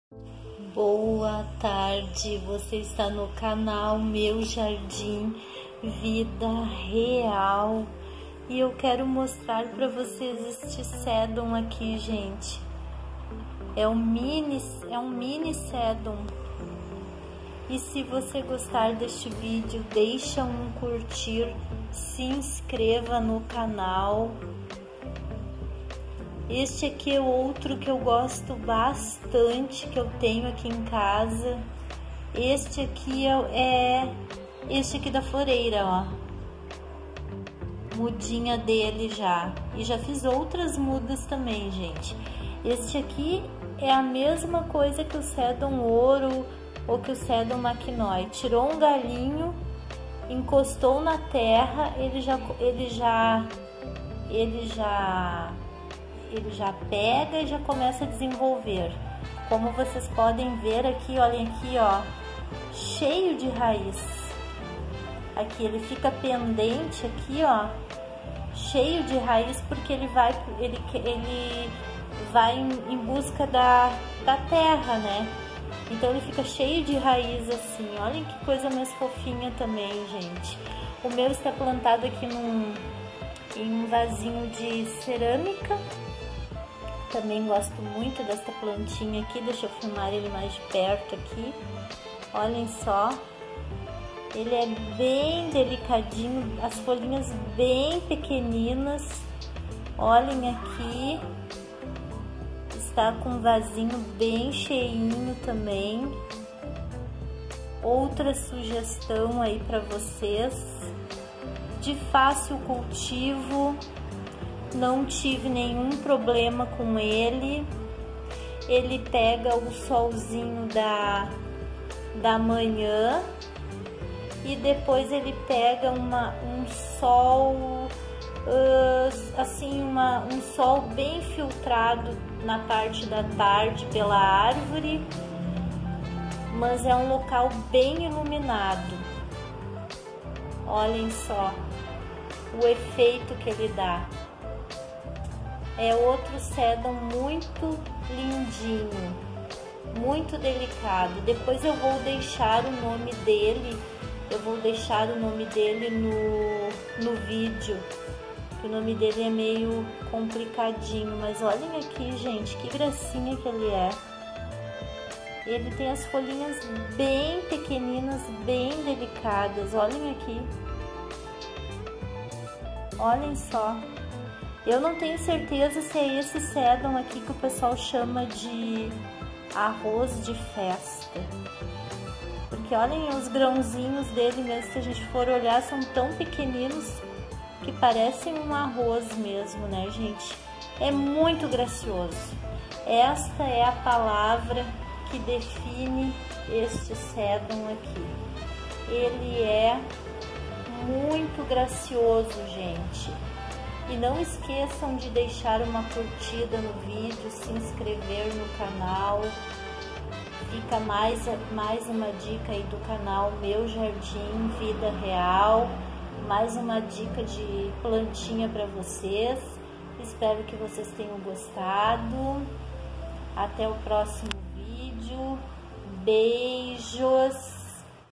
Sedum Dasyphyllum, lindo, de fácil cultivo e de propagação rápida!
Um sedum maravilhoso, lindo e de fácil cultivo, propaga muito facilmente.